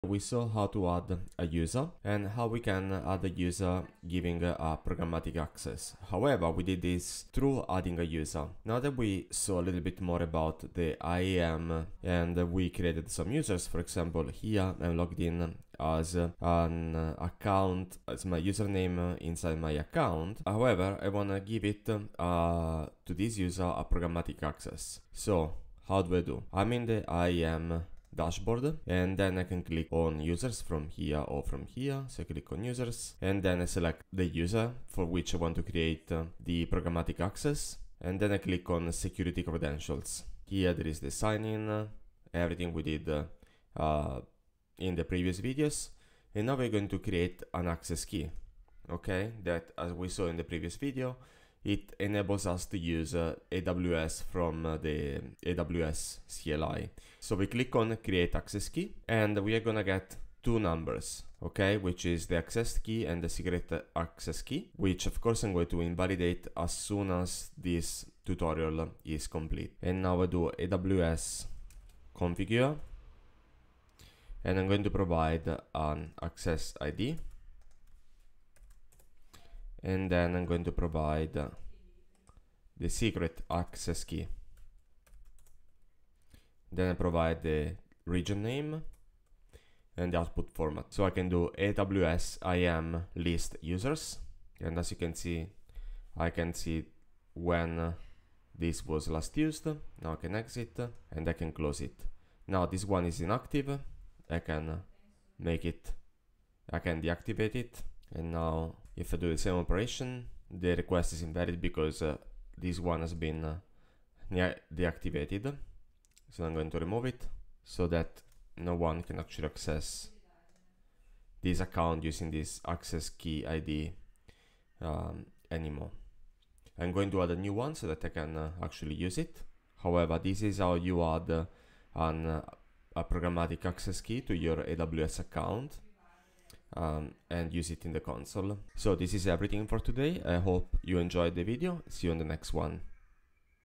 How to add programmatic access to existing aws user
How can we enable user access via the CLI/SDK to users we added to the IAM system?
If you followed my video about how to add users and generate access keys - you might want to be able to perform the same on another user that already exists and this video shows how to.